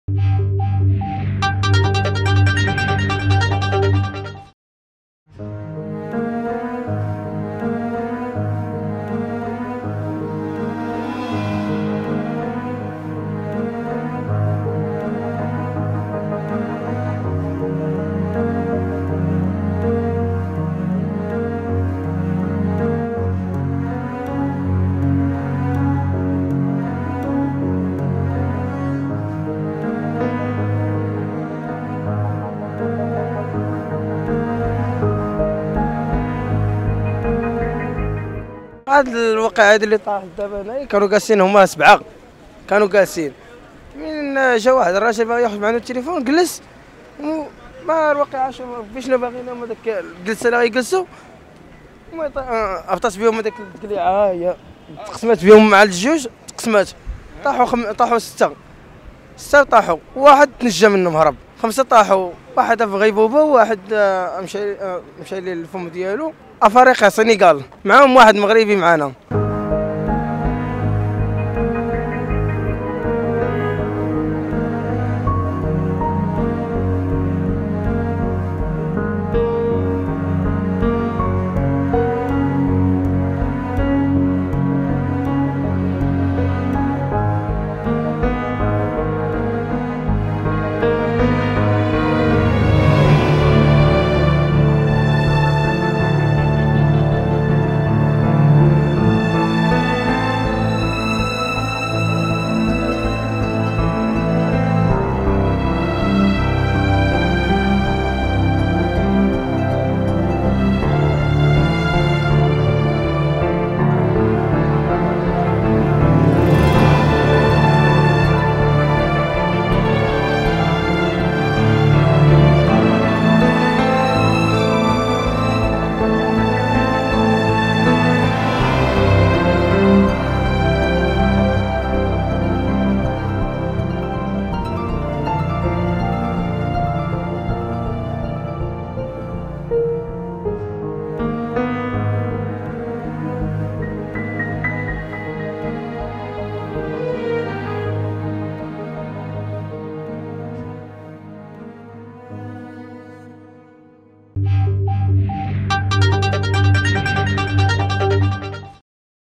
لحظة نقل 5مهاجرين ومغربي إلى المستشفى بعد سقوطهم من الكرة الأرضية بالبيضاء
شهد ممر "الكرة الأرضية" بساحة ماريشال بمدينة الدار البيضاء،  يومه الأربعاء وقوع حادث خطير، يتمثل في سقوط ثلاث مهاجرين من دول إفريقيا جنوب الصحراء، ومواطن بداخل "الممر".